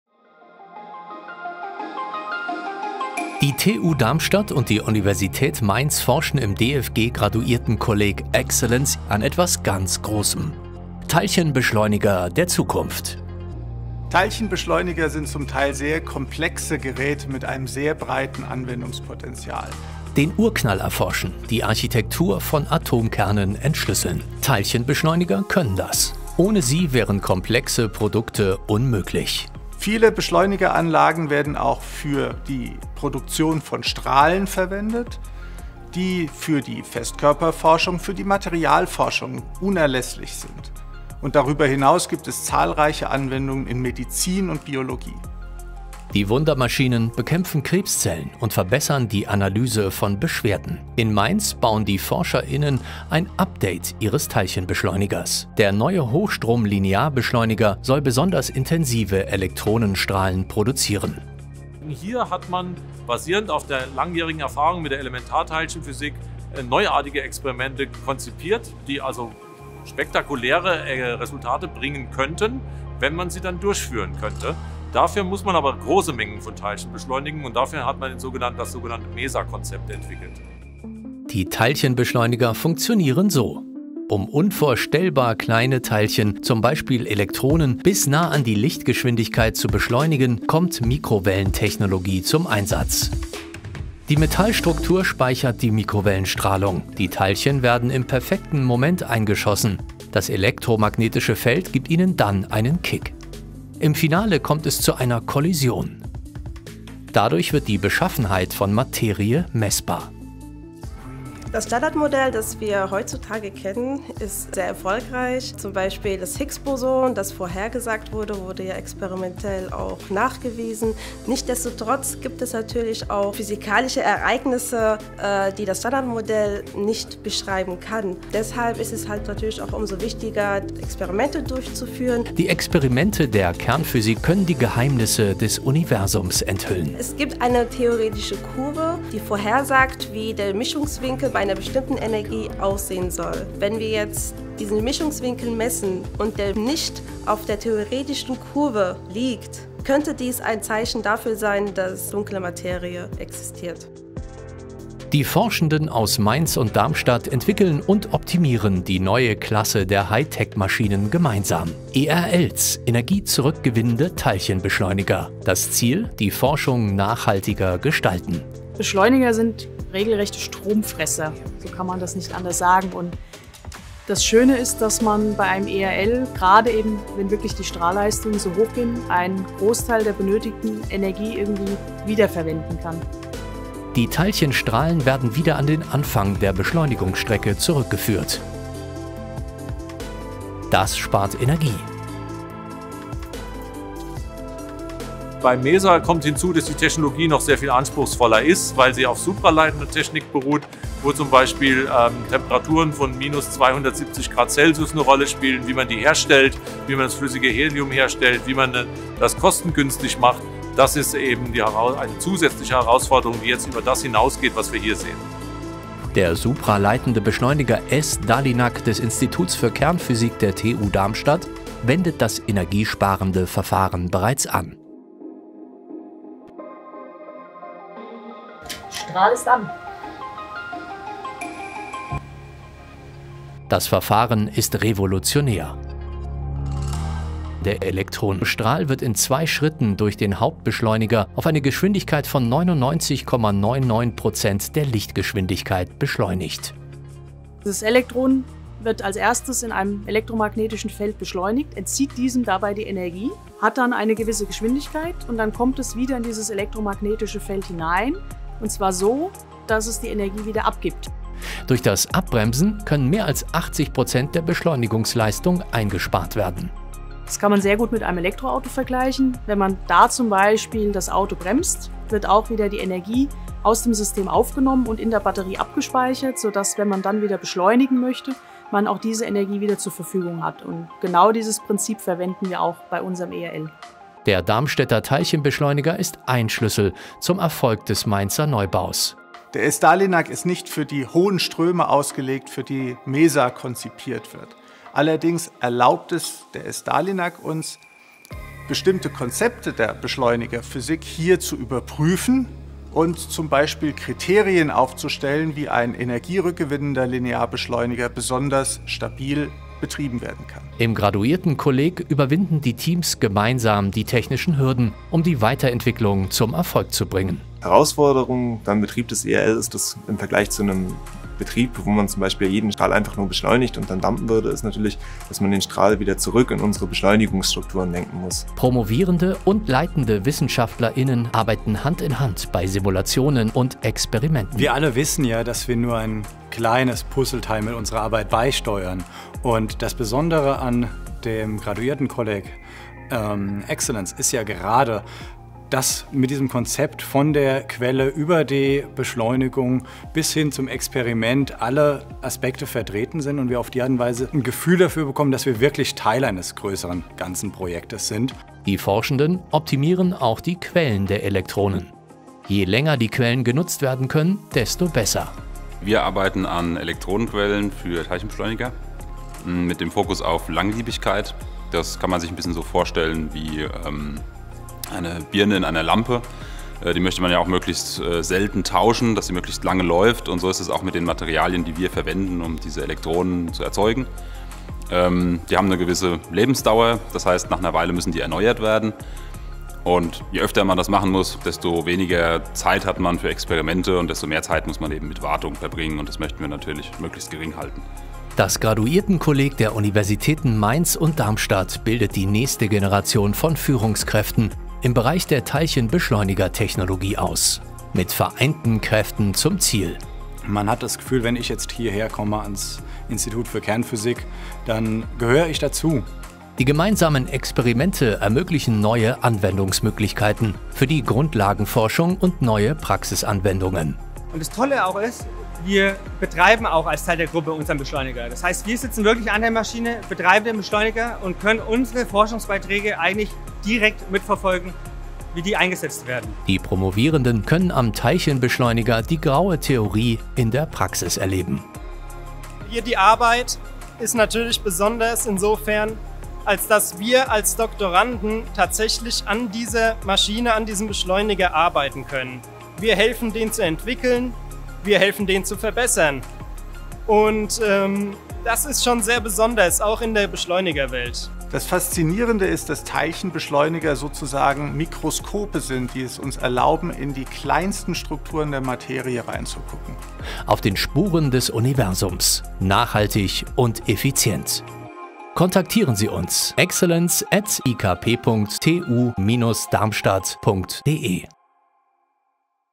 AccelencE: Forschung am Teilchenbeschleuniger der Zukunft
Die TU Darmstadt und die Uni Mainz forschen im DFG-Graduiertenkolleg AccelencE gemeinsam an einem EnergyRecoveryLinac-Teilchenbeschleuniger der Zukunft.